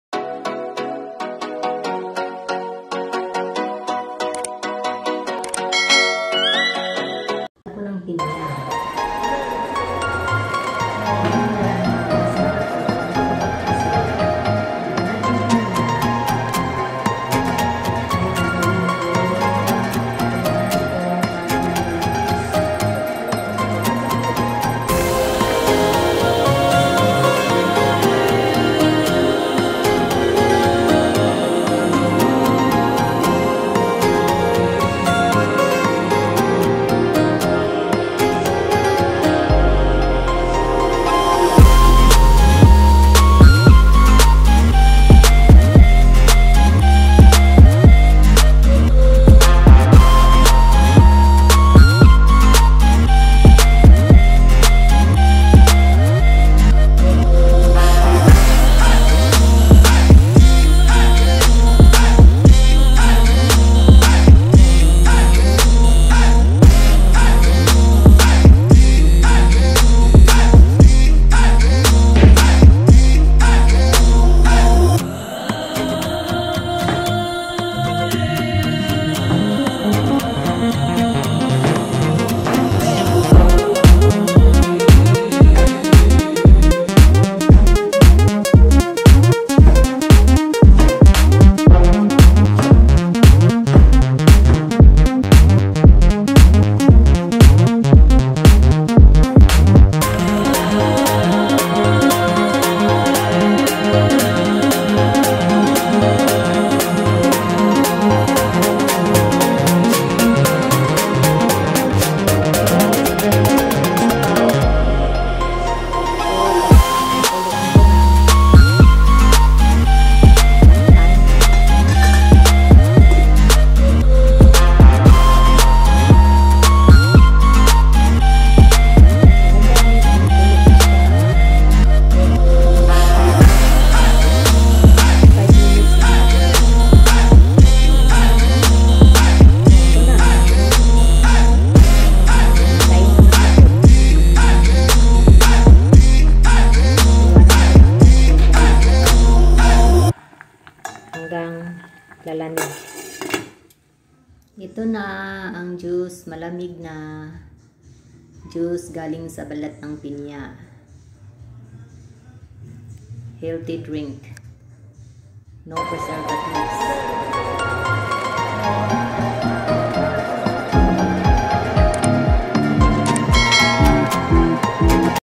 HOW TO MAKE PINEAPPLE JUICE USING PINEAPPLE PEEL
Clean your pineapple peels thoroughly and place in a large pot with enough water to cover them.. let them boil for about 10 minutes. Once ready, add your sweetened and refrigerate it for about 30 minutes. Serve with some ice cubes or mint leaves.  Just like a fruit, the skin is rich in Vitamin C, which can build the overall immunity of the body, fight bacteria, help with cough, etc. You can also use it to marinate your meat.